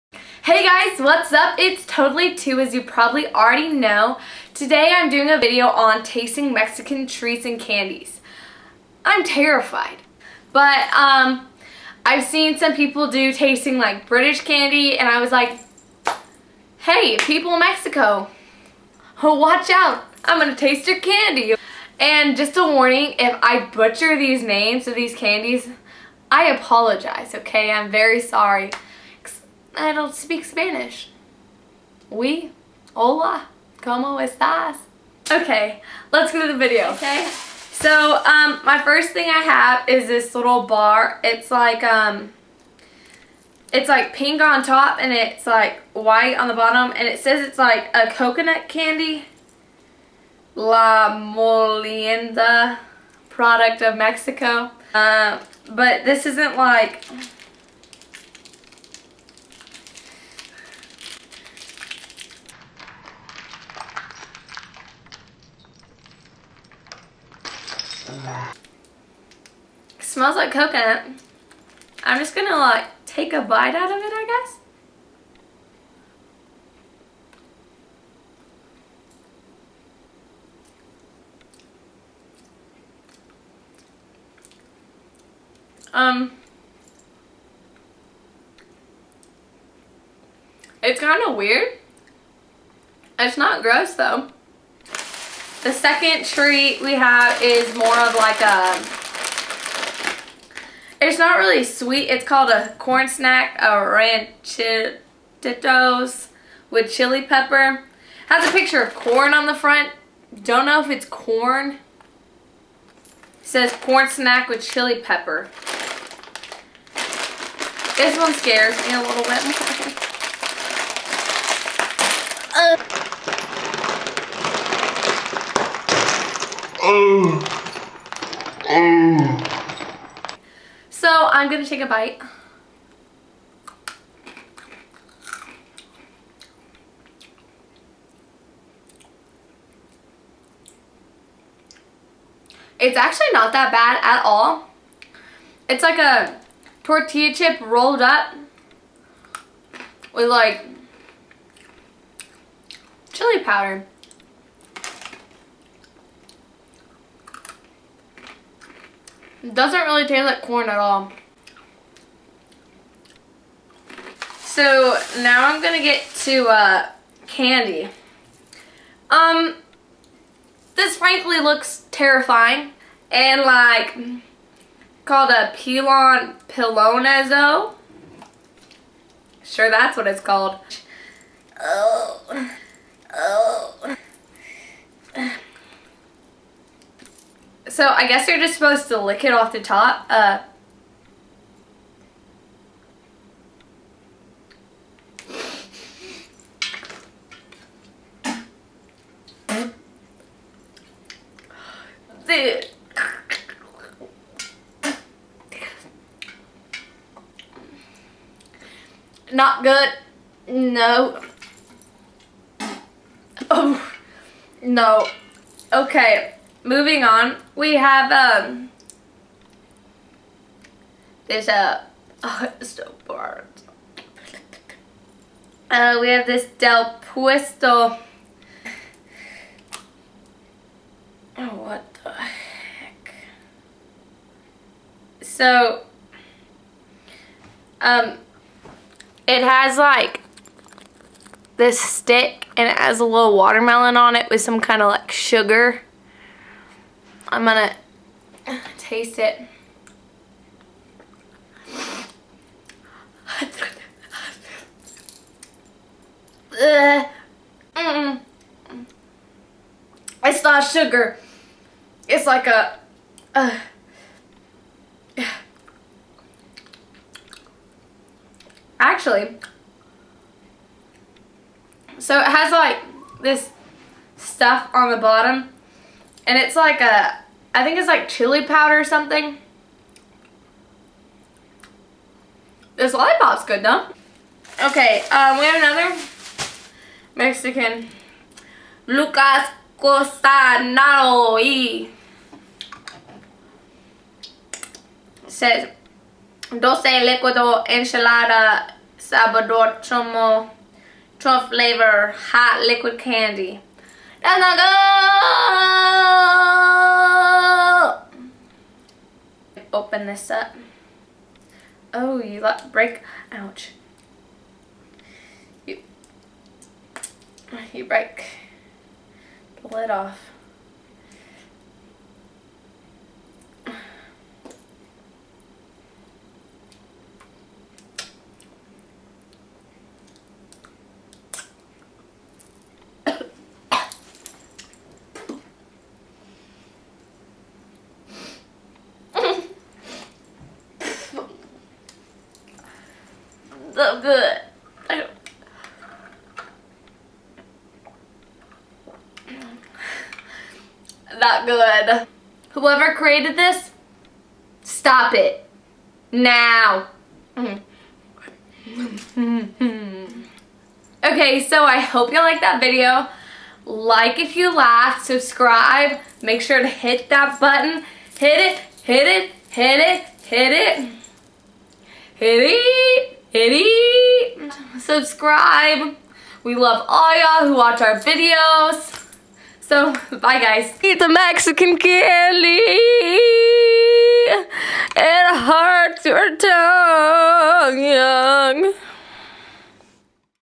Tasting Mexican Treats & Candy | Totally Two
I had so much fun filming tasting Mexican treats and candy for y'all!!! I hope y'all enjoy this video and it makes you smile :))))

See y'all next Tuesday ;))))